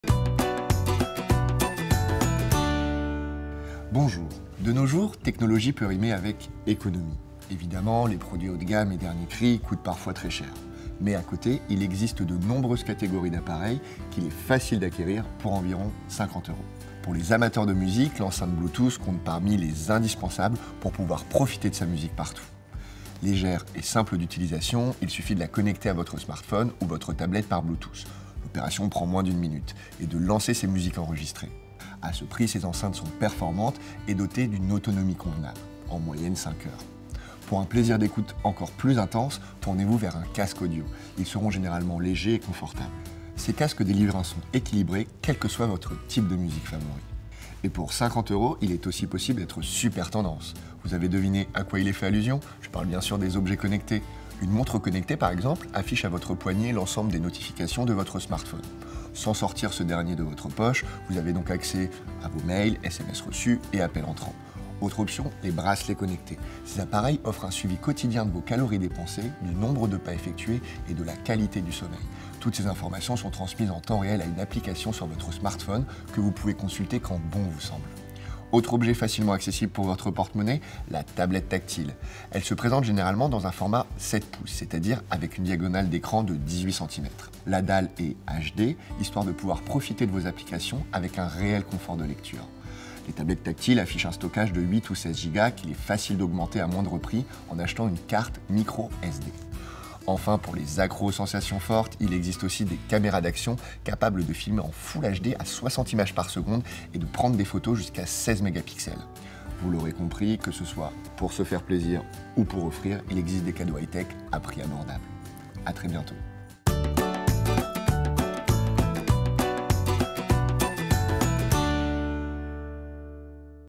C’branché – Idées cadeaux high-tech à moins de 50 €
Des cadeaux high-tech à prix abordables
Enceinte Bluetooth, casque audio, montre et bracelet connectés, tablette tactile, caméra d’action… notre journaliste vous présente une sélection d’appareils high-tech à moins de 50 €. Oui, « technologie » peut rimer avec « économie » : regardez !